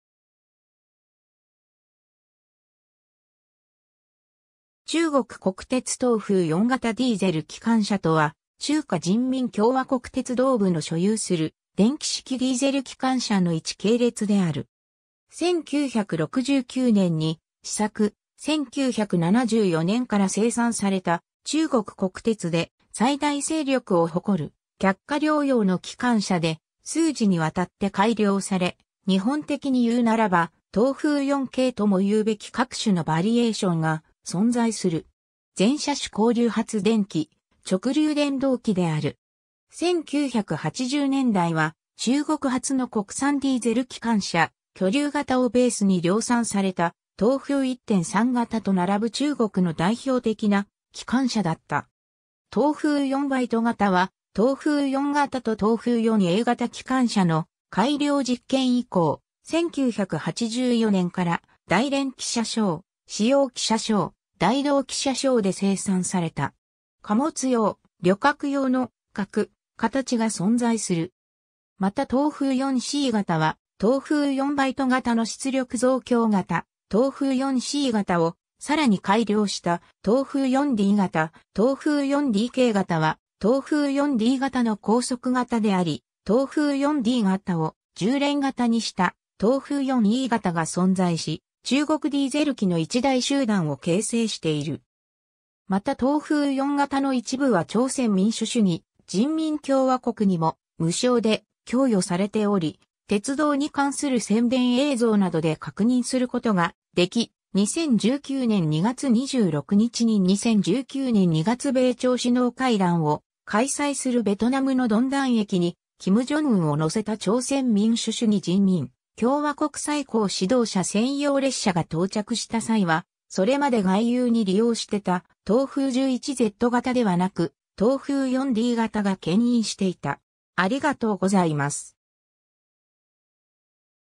中国国鉄東風4型ディーゼル機関車
中国国鉄東風4型ディーゼル機関車（ちゅうごくこくてつとうふう4がたディーゼルきかんしゃ）とは、中華人民共和国鉄道部の所有する電気式ディーゼル機関車の1系列である。
 1969年に試作、1974年から生産された中国国鉄で最大勢力を誇る客貨両用の機関車で、数次に渡って改良され、日本的にいうならば「東風4系」ともいうべき各種のバリエーションが存在する。
全車種交流発電機、直流電動機である。
1980年代は中国初の国産ディーゼル機関車「巨龍型」をベースに量産された東風1・3型と並ぶ中国の代表的な機関車だった。
 東風4B型は東風4型と東風4A型機関車の改良実験以降、1984年から大連機車廠、資陽機車廠、大同機車廠で生産された。
貨物用、旅客用の各形が存在する。
また東風4C型は東風4B型の出力増強型、東風4C型をさらに改良した東風4D型、東風4DK型(3000系)は東風4D型の高速型であり、東風4D型を重連型にした東風4E型が存在し、中国ディーゼル機の一大集団を形成している。
また東風4型の一部は朝鮮民主主義人民共和国にも無償で供与されており、鉄道に関する宣伝映像などで確認することができ、2019年2月26日に2019年2月米朝首脳会談を開催するベトナムのドンダン駅に金正恩を乗せた朝鮮民主主義人民共和国最高指導者専用列車が到着した際はそれまで外遊に利用してた東風11Z型ではなく、東風4D型が牽引していた。